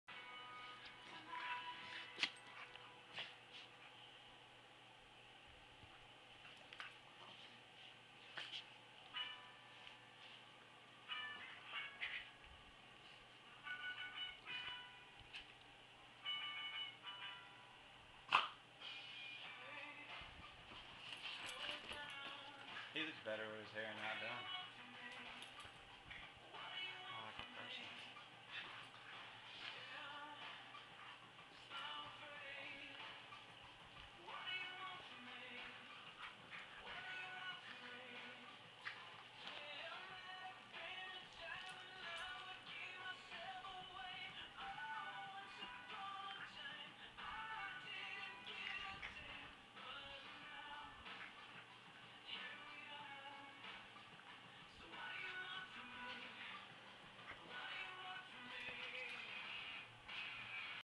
Bailey my min pin and Max my Doberman Playing 001.avi
This is a video of my Doberman Max playing with my min pin Bailey.  It's really short because I took it with my digital camera.